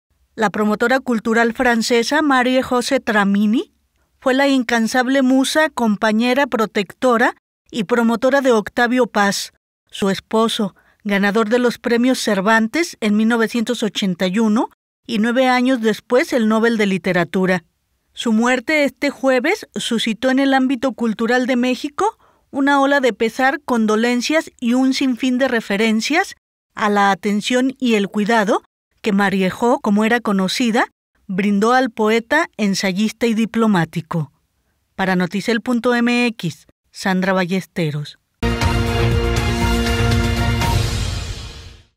Muere compañera de Octavio Paz
Resalta mundo cultural, labor de Marie-Jo, como se conocía a la pintora francesa